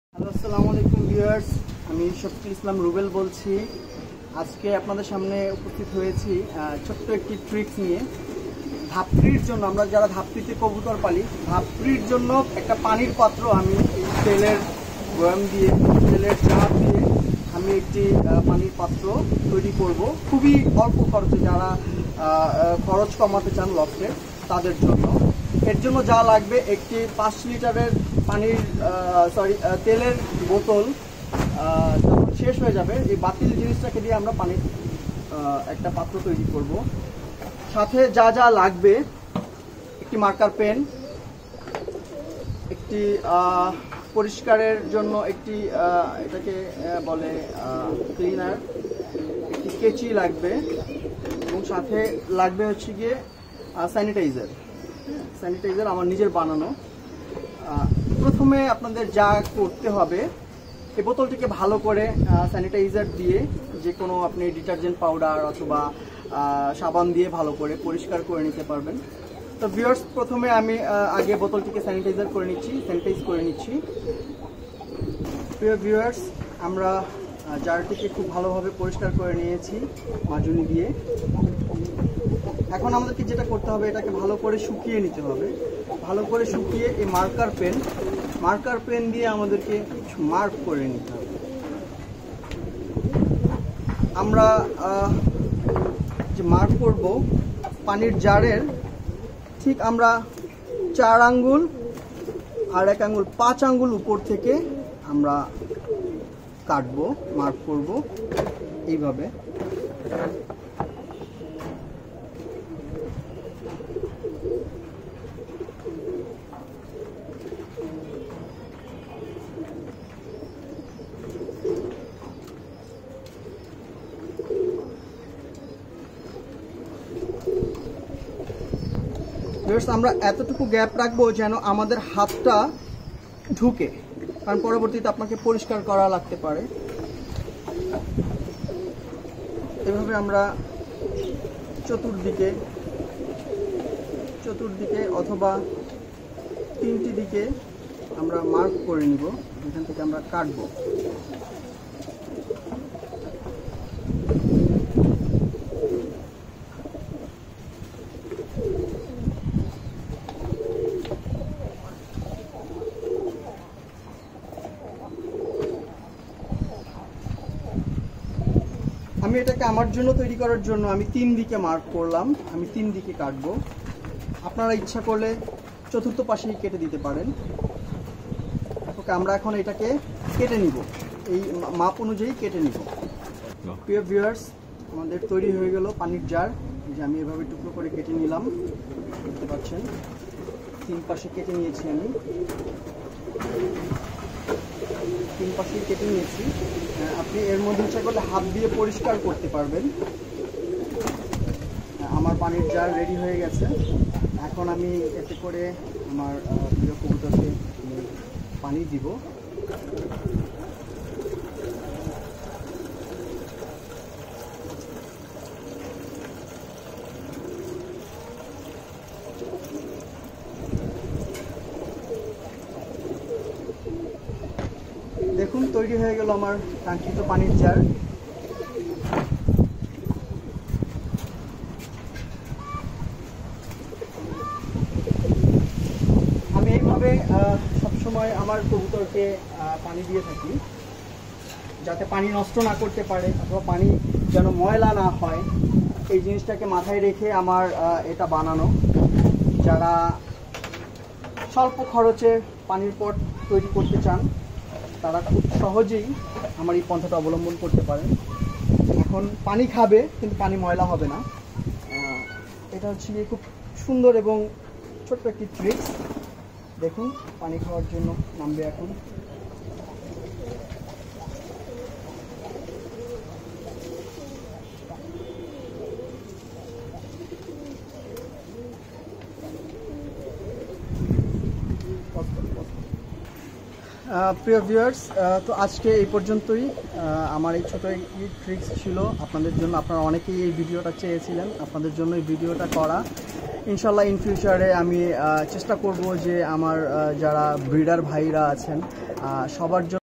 কবুতরের জন্য বিনা খরচে পানির পাত্র
বাতিল তেলের গ্যালন দিয়ে ধাপ্রির কবুতরের জন্য একটি পানির পাত্র তৈরির প্রস্তুত প্রনালী।